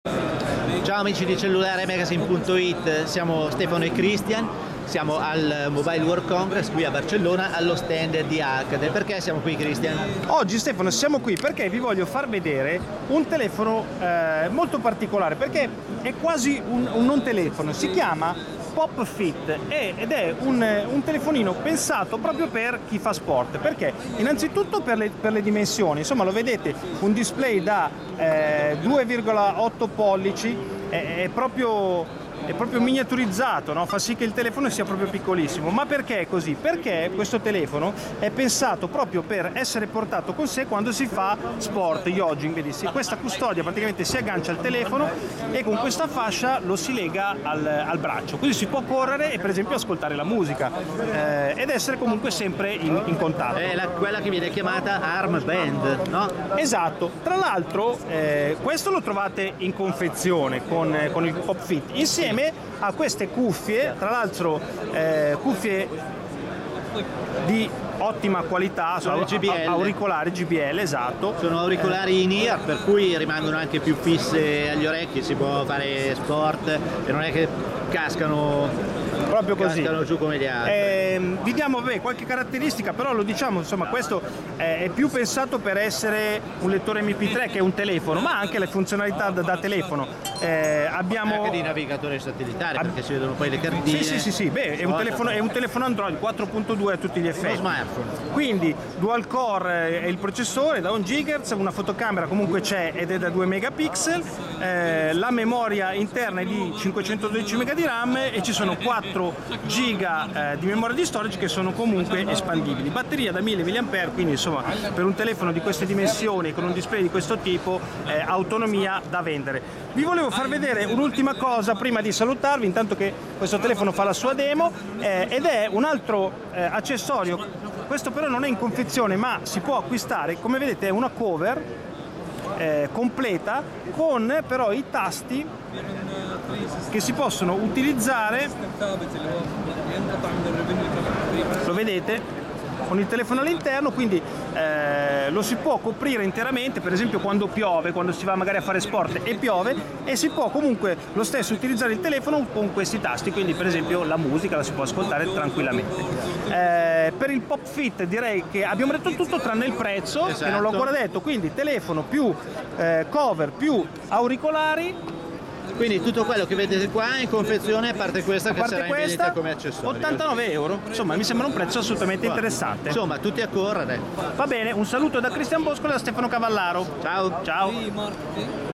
MWC2014: Preview Alcatel OT Pop Fit
Un telefonino molto originale, con dimensioni e display piccoli, pensato per chi pratica attività sportiva e abbinato a una fascia Arm Band